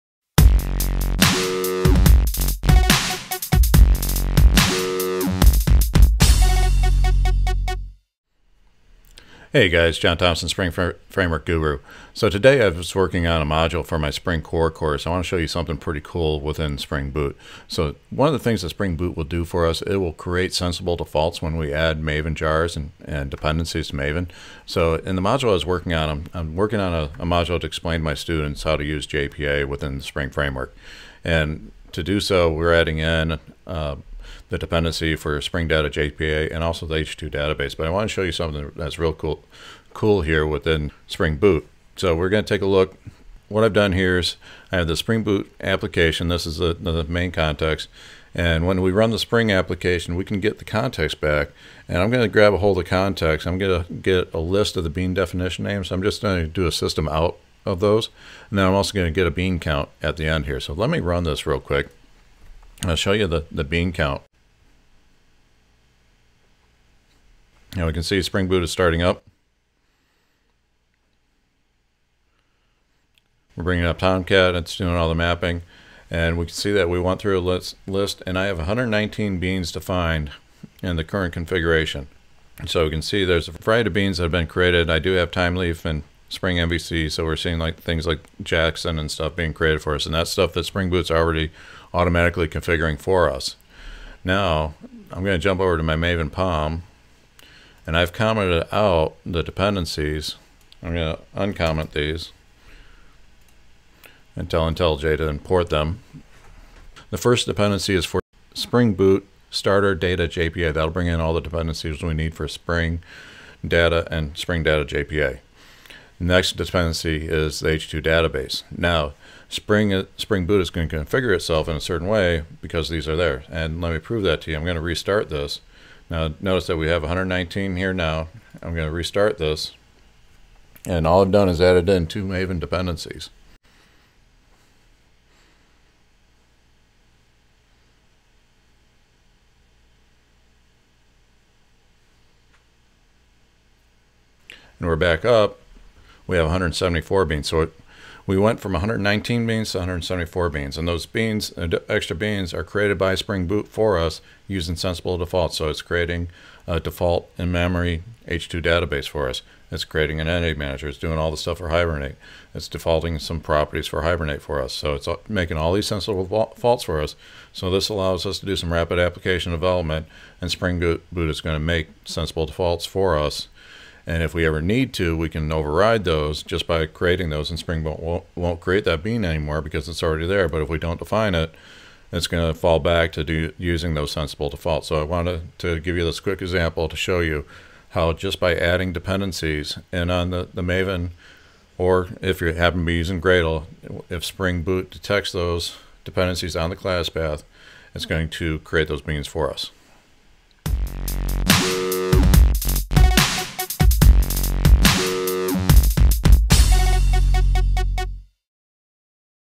Spring Boot Adding Spring Data JPA and H2
Spring Boot enables us to perform rapid application development by making sensible default choices when it finds certain classes on the classpath. In this example, I take an existing Spring Boot Application, and show you that when I add the Maven dependencies for Spring Data JPA and the H2 database, Spring Boot will create additional beans for JPA, Hibernate, and the H2 data source.